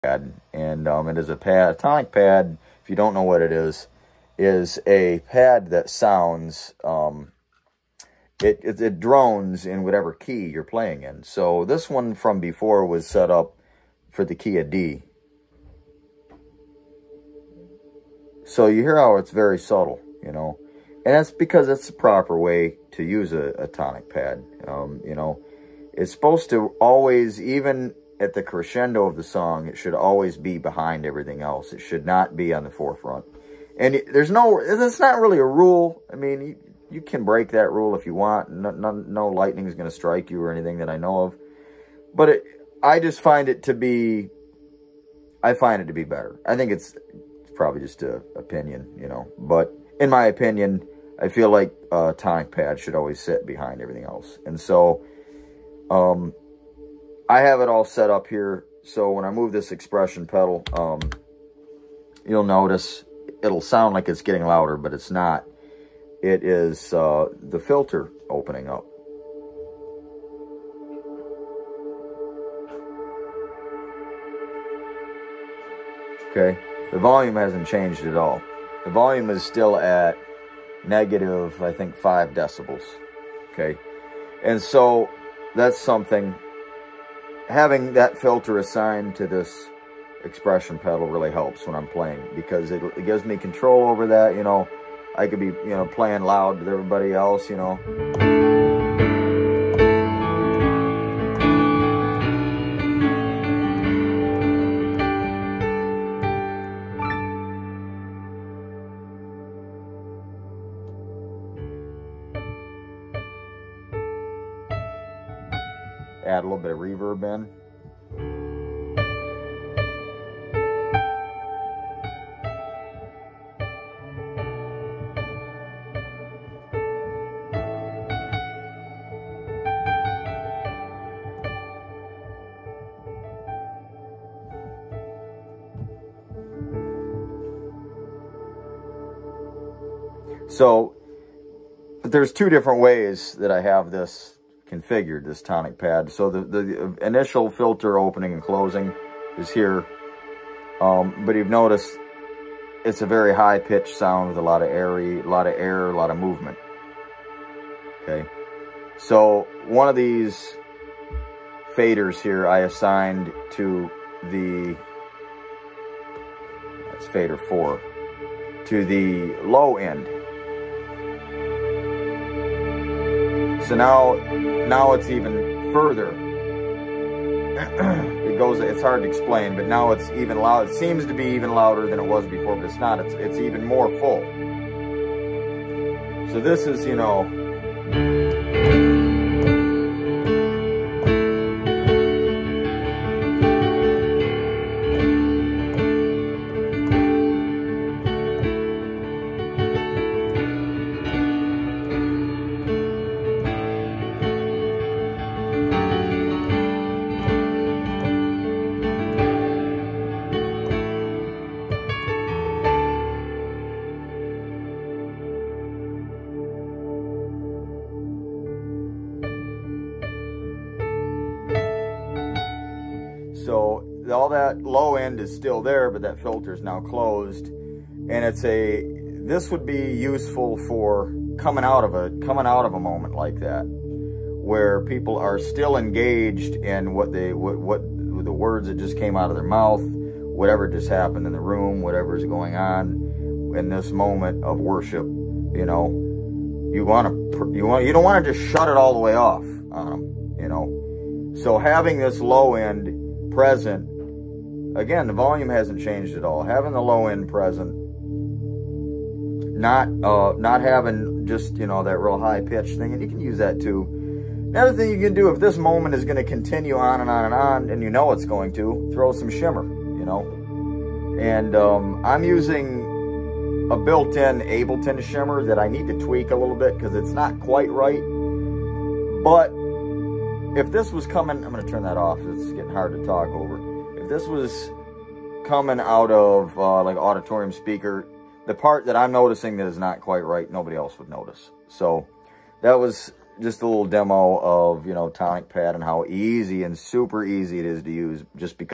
The use of tonic pads, general dynamics of worship.
TonicPads and how to use. Some general worship dynamics, utilization of filters, what controls to map said filters to.